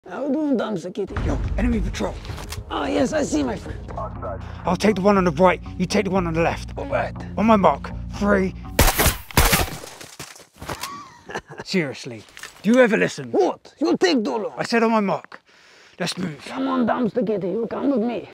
Enemy Patrol Down
Mobster and Artyom on a mission but Artyom like usual doesn't listen to the orders...

inspired loadout, Airsoft or cosplay, larp use.
This is for educational and Artistic purposes to show the sort of gear and equipment they use, featured in video games and movies.